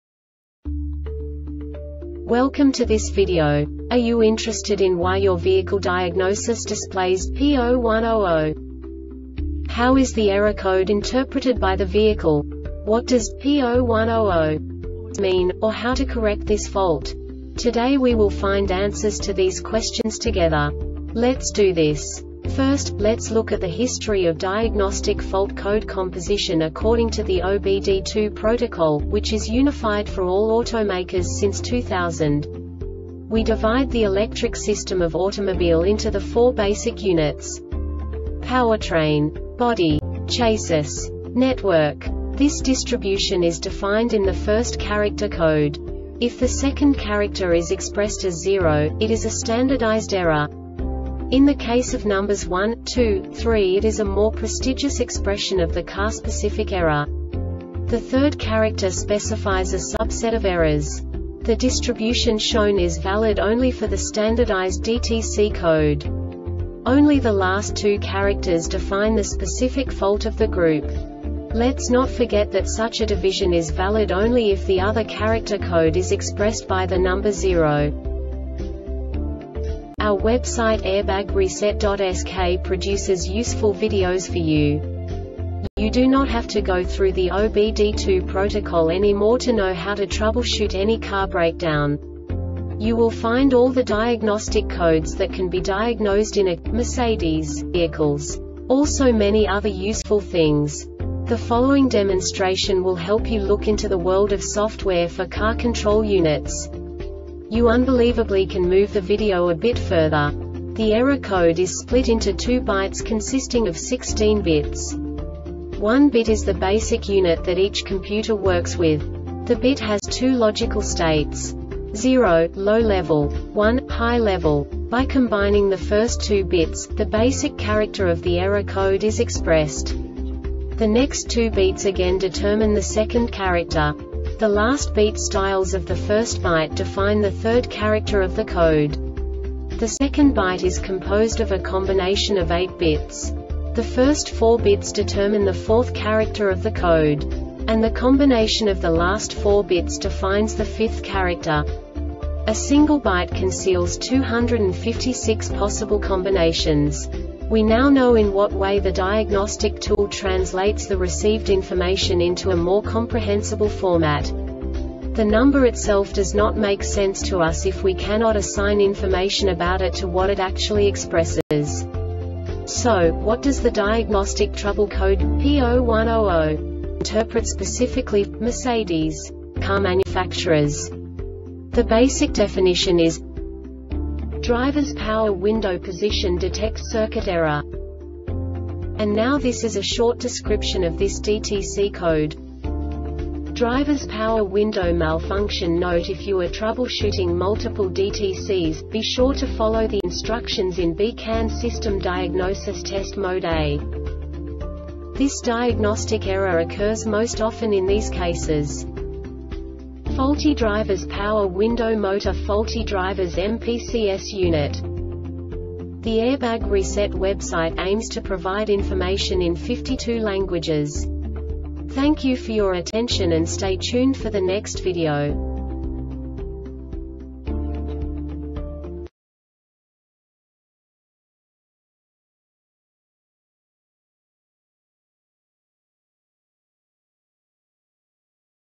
DTC Mercedes-Benz P0100 Short Explanation
Contents:
0:21 Basic DTC analysis according to OBD2 protocol standard.
1:48 Insight into programming
2:58 A diagnostically understandable description of the Diagnostic Trouble Code
3:05 Basic error definition

Make
Mercedes Benz
Code
P0100
Definition
Driver's Power Window Position Detect Circuit Error
Description
Driver's Power Window malfunction  NOTE If you are troubleshooting multiple DTCs,  be sure to follow the instructions in B-CAN System Diagnosis Test Mode A
Cause
 Faulty Driver's Power Window Motor Faulty driver's MPCS unit